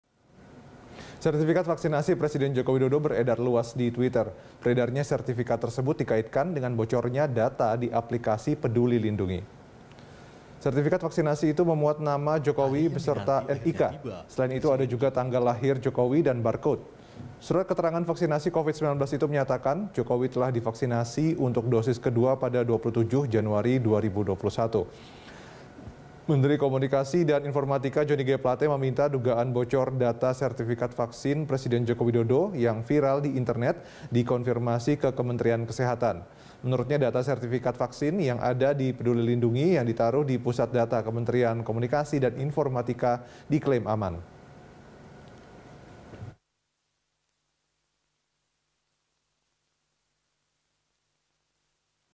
Sertifikat Vaksinasi Presiden Jokowi Bocor di Internet?
Sertifikat vaksinasi Presiden Joko Widodo beredar luas di Twitter. Beredarnya sertifikat tersebut dikaitkan dengan bocornya data di aplikasi Pedulilindungi.